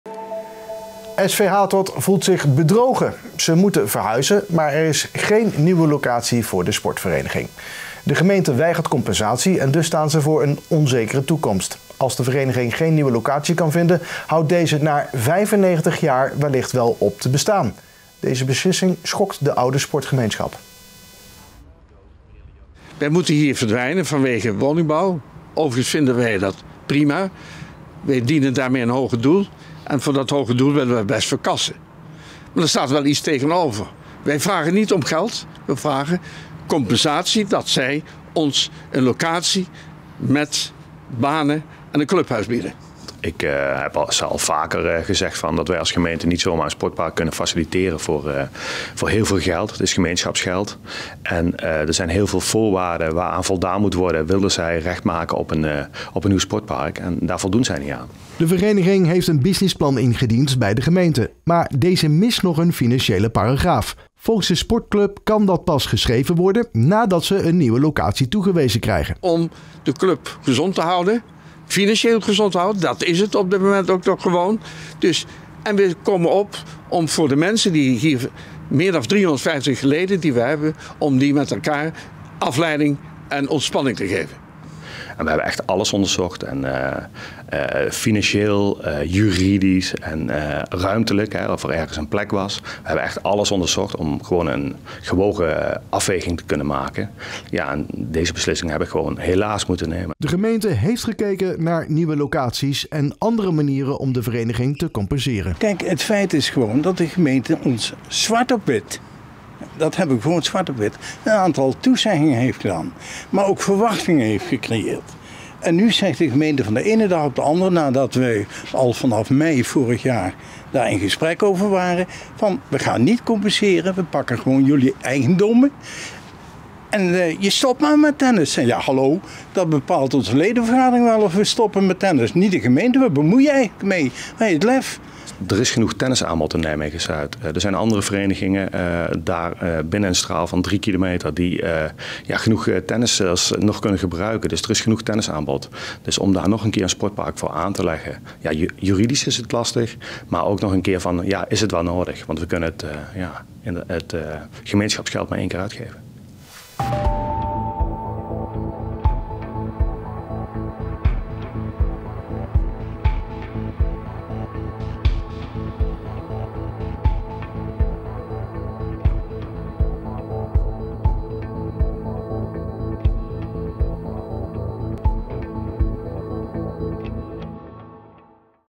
Geen plaats meer voor SV Hatert || RN7 REGIONIEUWS
SV Hatert voelt zich bedrogen; ze moeten verhuizen, maar er is geen nieuwe locatie voor de sportvereniging. De gemeente weigert compensatie en dus staan ze voor een onzekere toekomst. Als de vereniging geen nieuwe locatie kan vinden, houdt deze, na 95-jaar, wellicht wel op te bestaan. Deze beslissing schokte de oude sportgemeenschap.